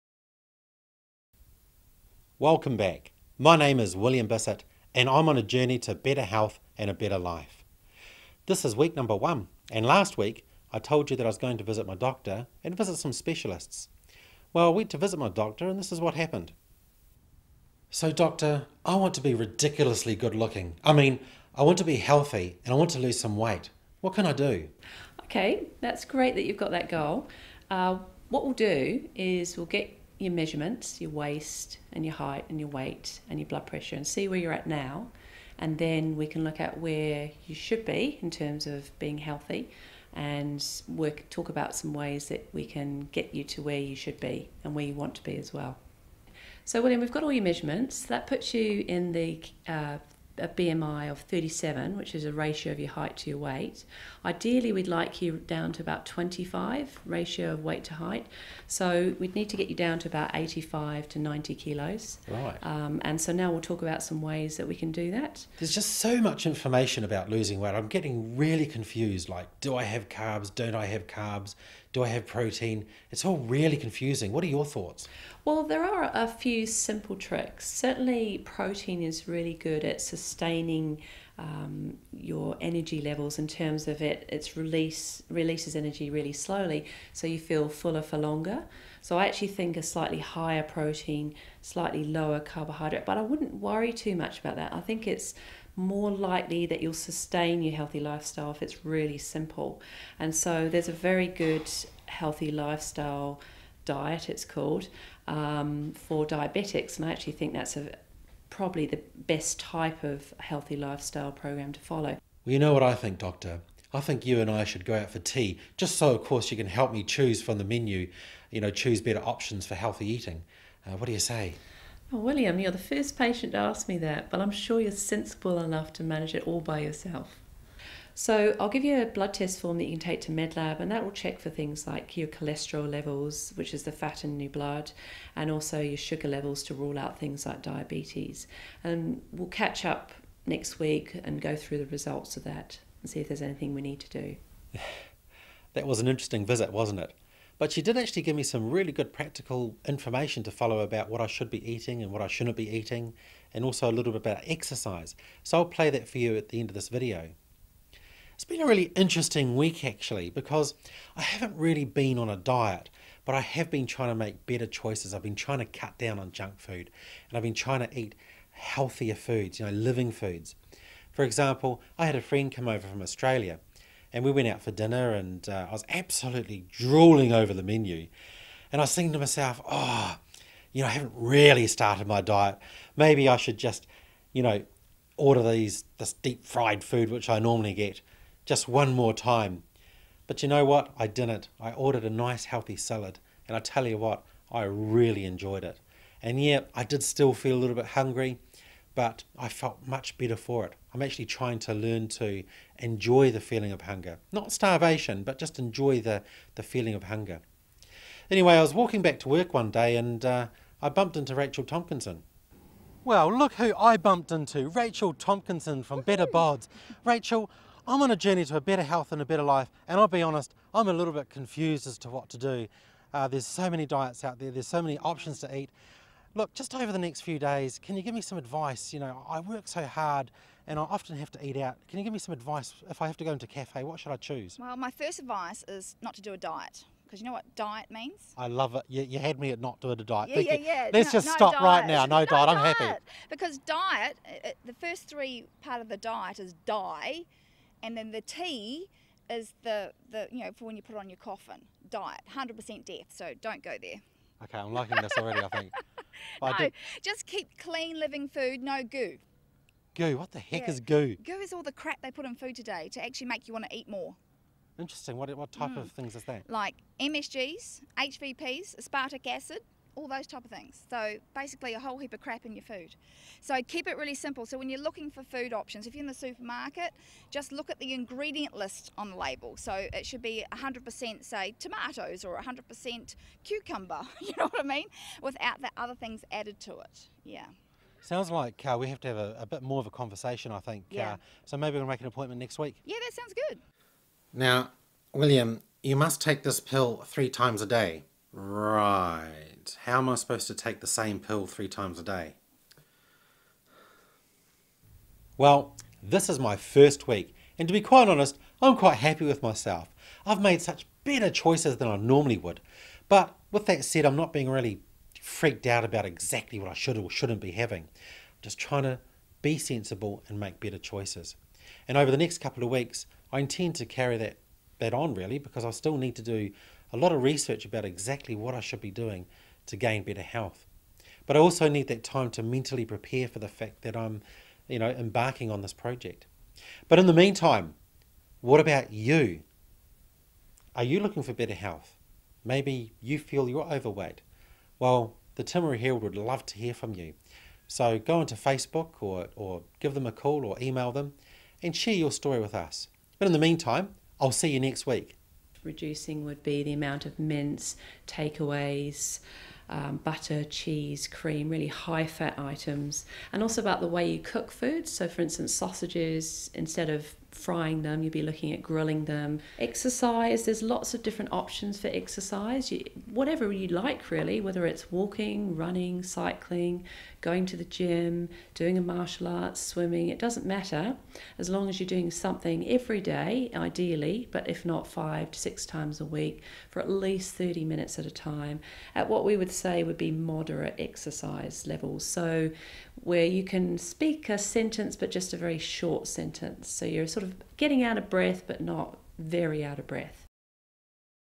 #02 - How to lose weight the funny way with William Bisset
William Bisset from Timaru is on a Journey to 'Better Health & a Better Life". It is a semi-comedy series with a touch of naughtiness.